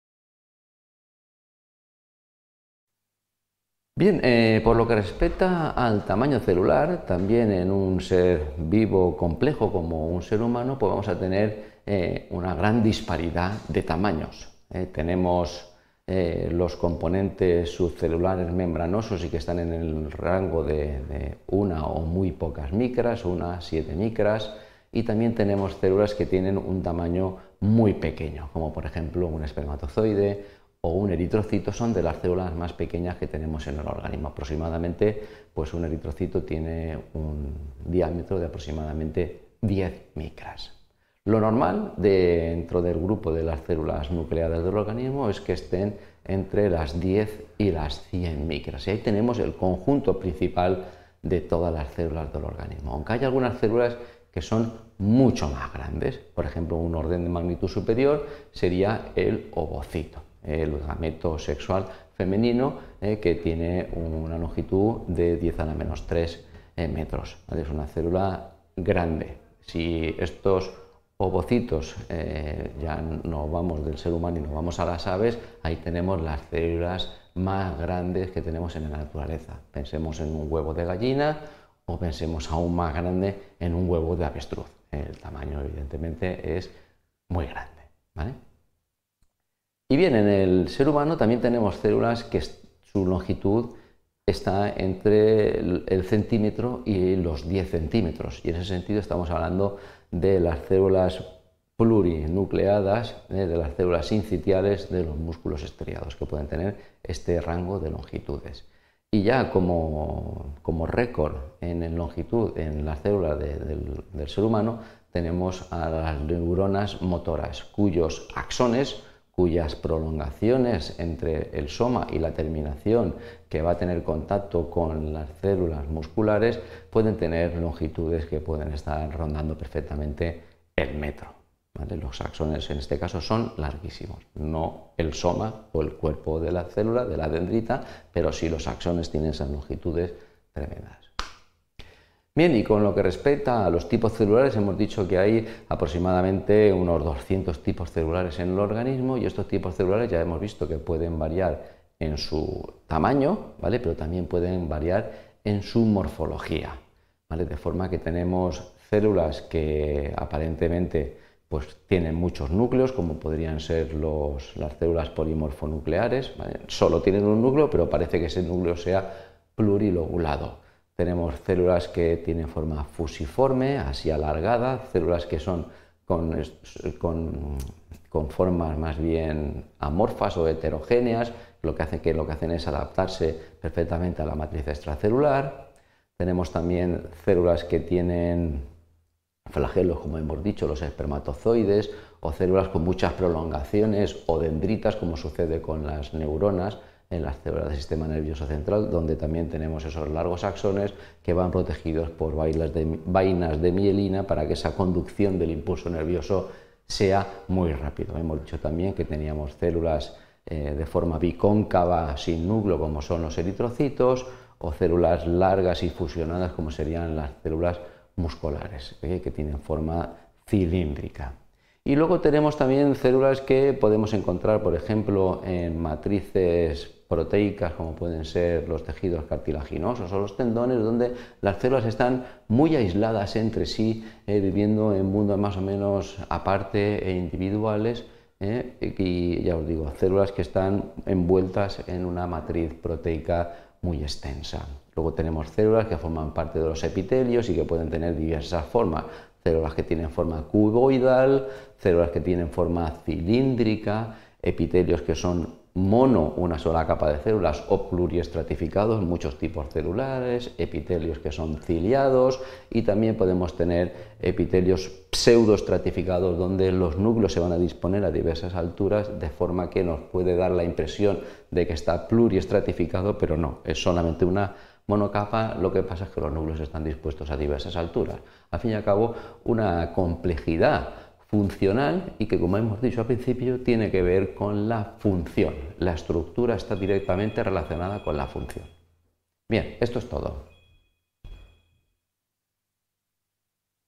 El tamaño y la forma celular | 5/60 | UPV
Título: El tamaño y la forma celular

Descripción automática: En este video, se explica la variabilidad en tamaño y morfología de las células en un organismo humano. Se mencionan estructuras subcelulares membranosas y se describen células pequeñas como espermatozoides y eritrocitos, con diámetros cercanos a las diez micras. Las células nucleadas habituales oscilan entre 10 y 100 micras, aunque hay excepciones como el ovocito, e incluso células aviares más grandes.

Se habla de células plurinucleadas y las incipientes de músculos estriados que pueden alcanzar hasta diez centímetros. Las neuronas motoras se destacan por sus axones que pueden llegar a un metro de longitud.

La diversidad celular incluye unos doscientos tipos con formas variadas, como células polimorfonucleares con núcleos plurilobulados, fusiformes, amorfos o con flagelos. Se resalta la adaptabilidad morfológica de las células al entorno extracelular y se mencionan dendritas y axones protegidos por mielina para la rápida conducción nerviosa. Se describen eritrocitos bicóncavos y células musculares cilíndricas, así como células en matrices proteicas como el cartílago y tendones.

Finalmente, se aborda la diversidad de células epiteliales en términos de forma y estratificación, enfatizando la relación directa entre la estructura y función celular.

Autor/a: Sirera Pérez Rafael